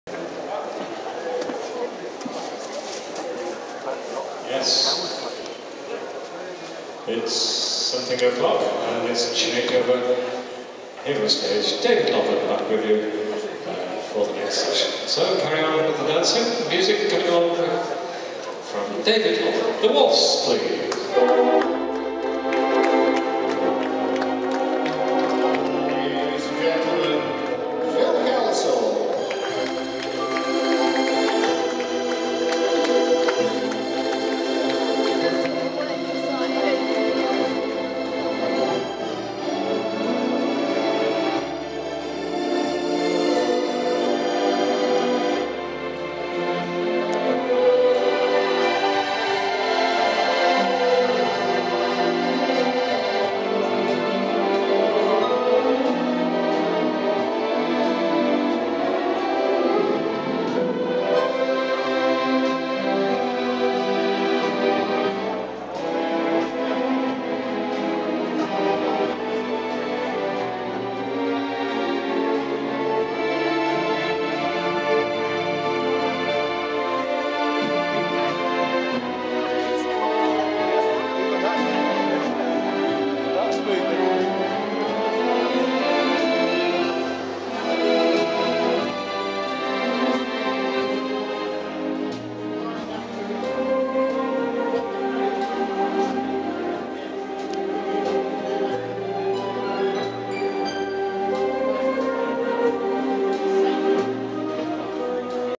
Phil Kelsall on the Mighty Wurlitzer at changeover time
Arrived at the Tower Ballroom just at the end of Phil's set and in time to see the Mighty Wurlitzer disappearing and the other, smaller keyboard, come forward. Didn't have the space for all that on my memory card but you see the Wurlitzer disappear at close quarters.